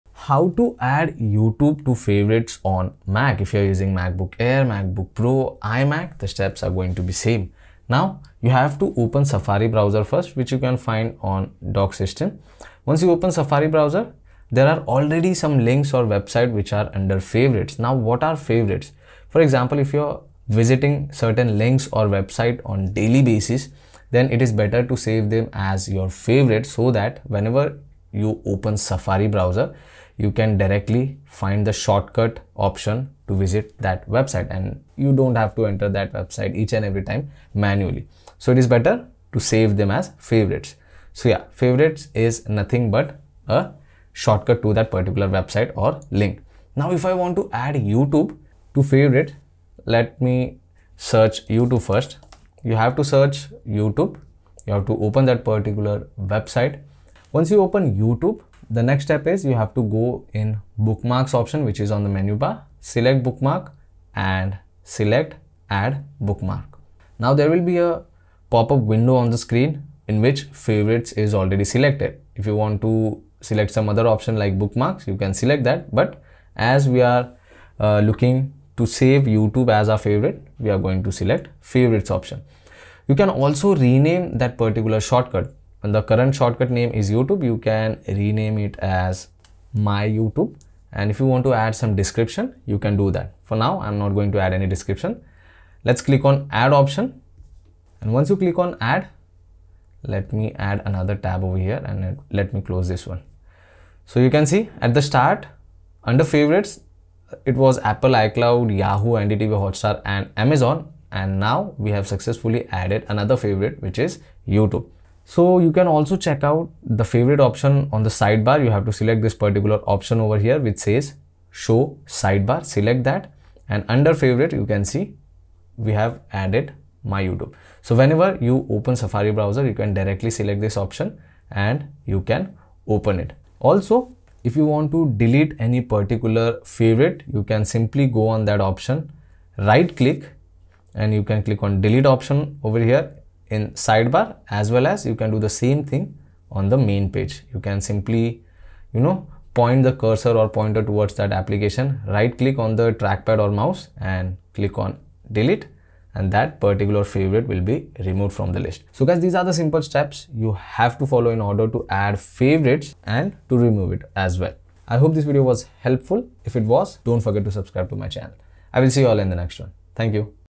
How to add YouTube to your favorites on Mac
How to add YouTube to your favorite in the Mac? Learn to use the steps and the way to add YouTube to your favorite in your MacBook Pro or Air. A simple tutorial that teaches you how you can add YouTube to your favorite in the Mac.